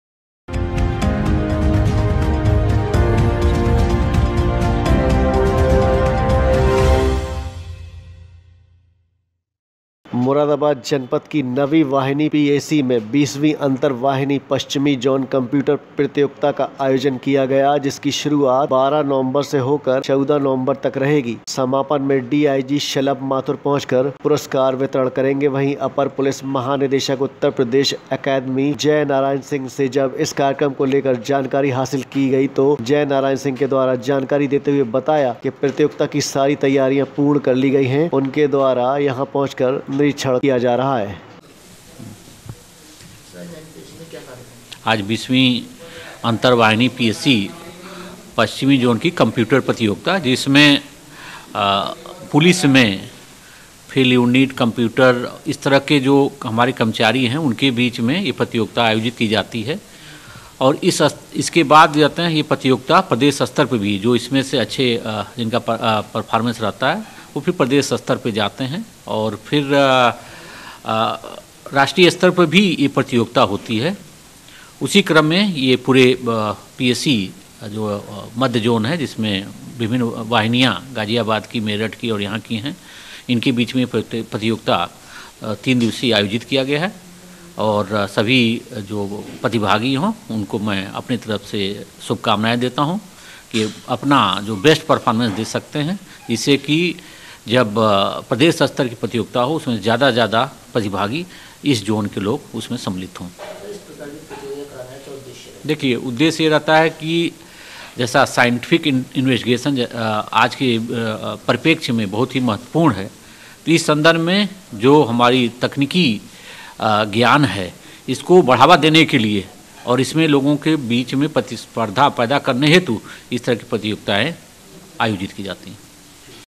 मुरादाबाद : जनपद में पीएसी में कार्यक्रम आयोजित।
उत्तर प्रदेश के मुरादाबाद जनपद के नबी वाहिनी पीएसी बीसवीं अंतर वाहिनी पीएसी पश्चिमी जून कंप्यूटर प्रतियोगिता का किया गया आयोजन 12 नवंबर से शुरू होकर 14 नवंबर तक कब चलेगा कार्यक्रम अपर पुलिस महानिदेशक उत्तर प्रदेश पुलिस अकैडमी में जय नारायण सिंह द्वारा किया गया कार्यक्रम का उद्घाटन 14 नवंबर को डीआईजी शलभ माथुर द्वारा पुरस्कार वितरण करने का कारक जाएगा कार्यक्रम जिसको लेकर तैयारी की गई पूरी।